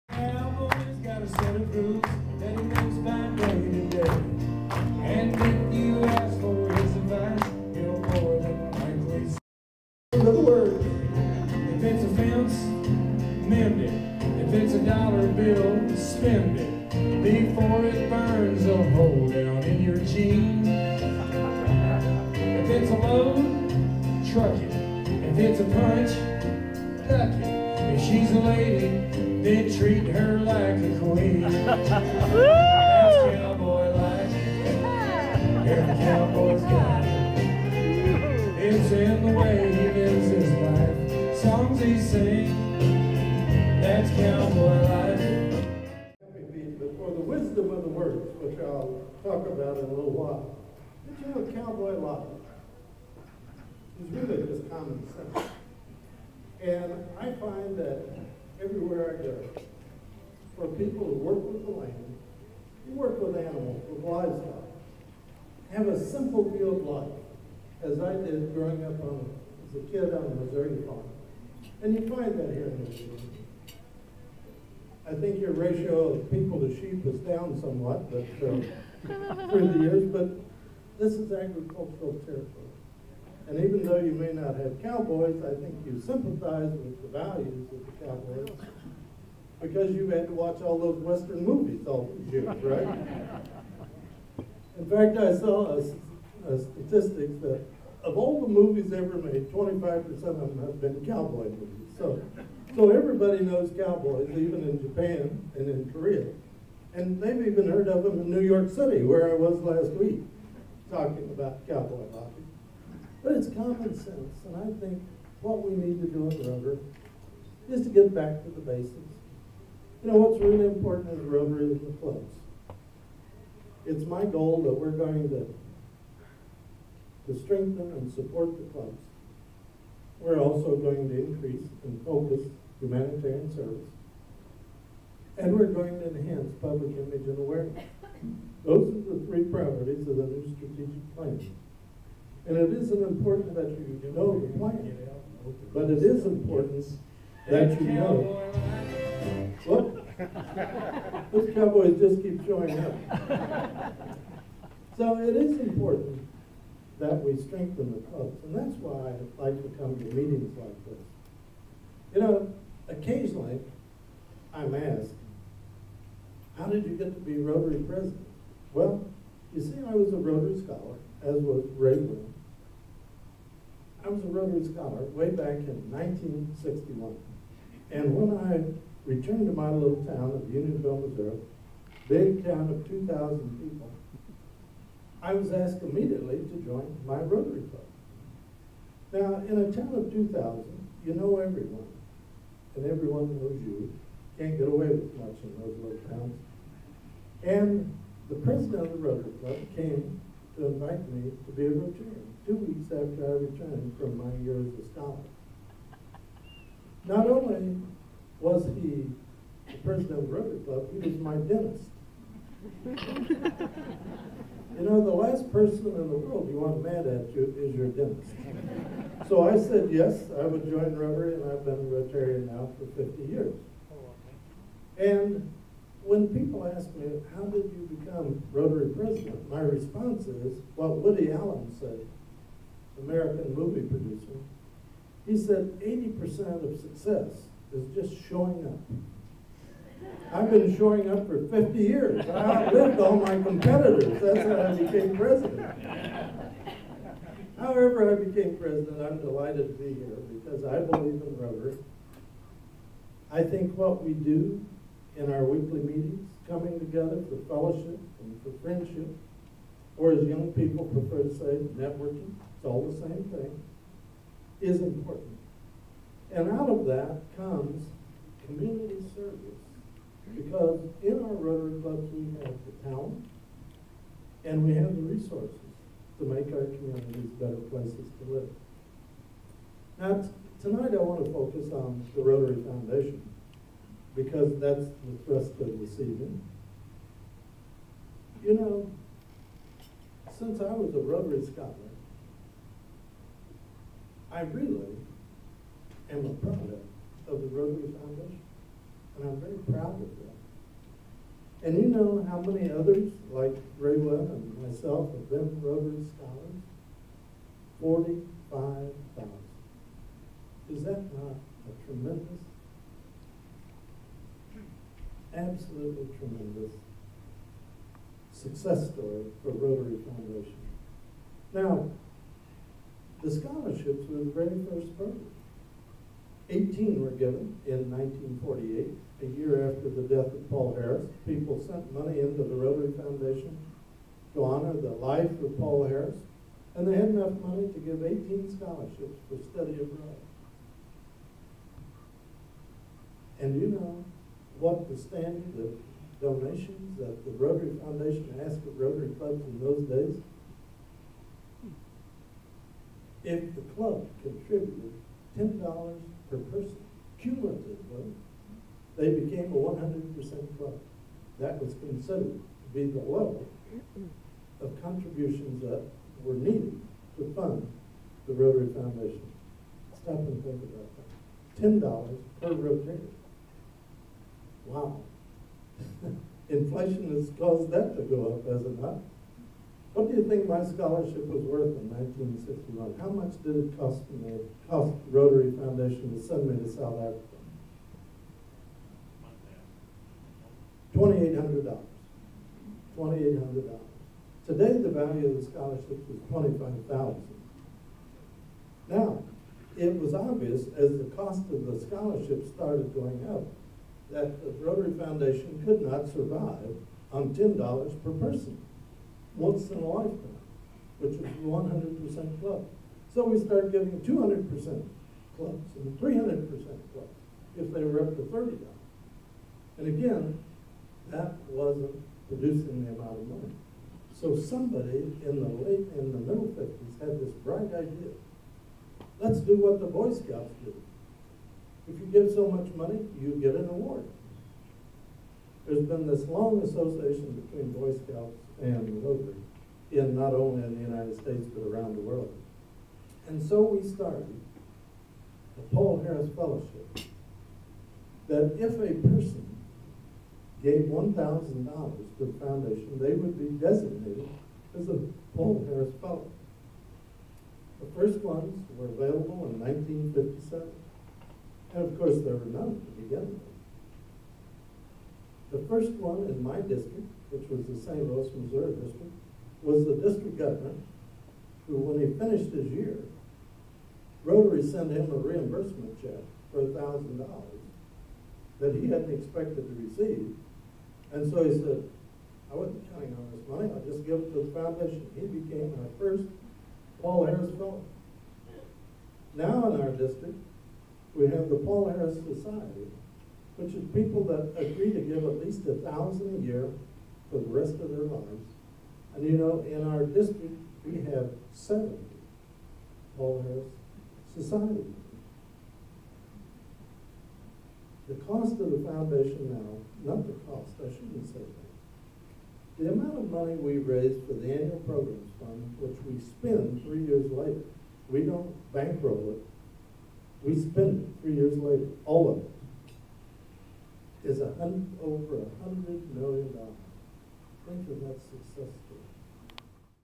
RI President Ray Klinginsmith
Just a PART of the Keynote Address at the Rotary Paul Harris Community Awards in Hamilton, NZ.